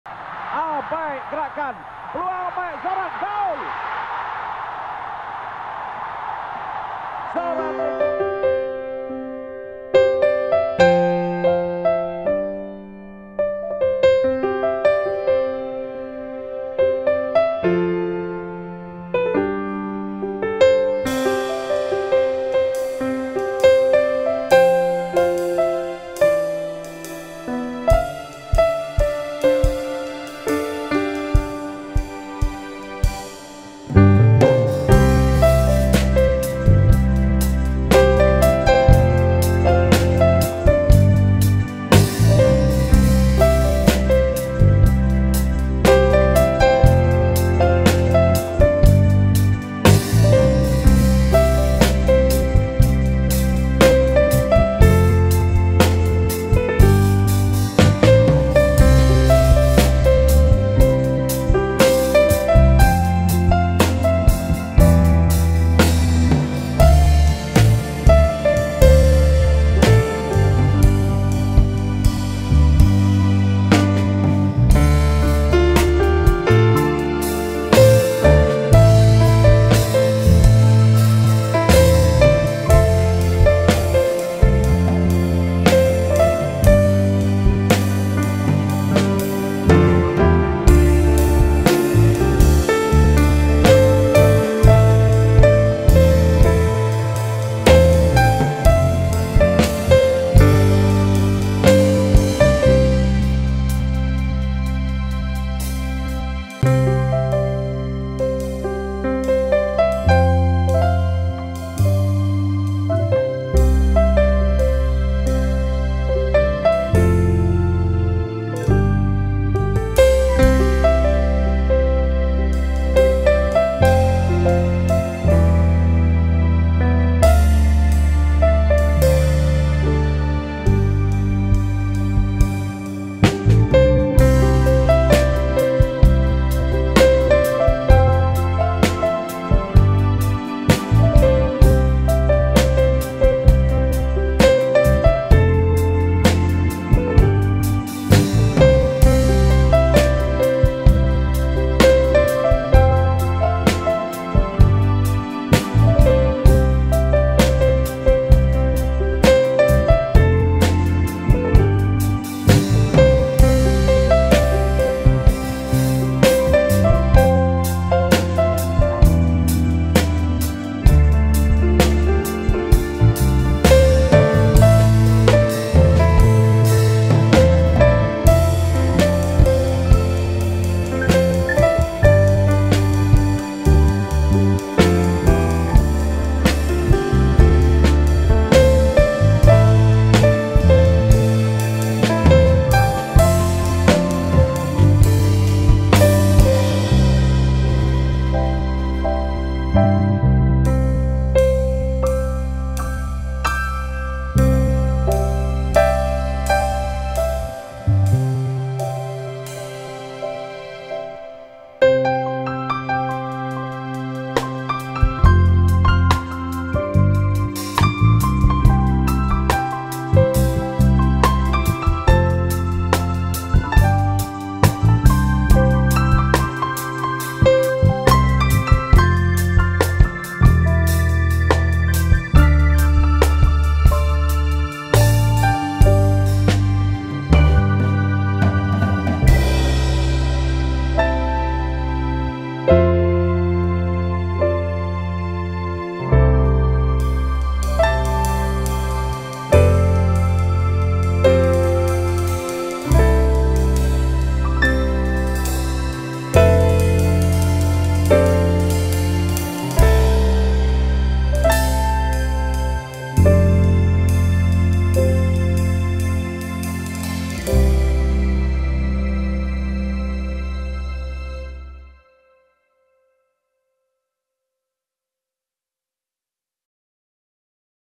After Bola Sepak, Apa Kerjaya Mereka?
Lagenda bola sepak 90-an, ada yang jadi Coach, ada yang bukak kedai, ada juga yang jadi Gelandangan. Jom Update tentang mereka